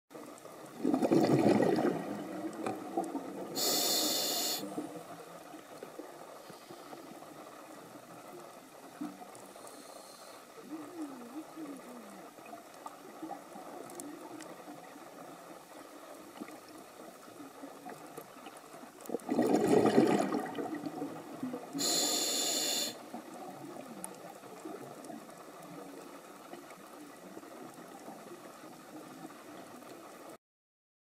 ソメンヤドカリ （’１９年９月 静岡県浮島）
’１９年９月　静岡県 浮島
背負った貝殻にいくつもイソギンチャクを乗せてる ソメンヤドカリ のペア…かな？
この前のイシダタミヤドカリもそうだけど、９月はヤドカリの恋の季節とかそういう感じなんですかね💓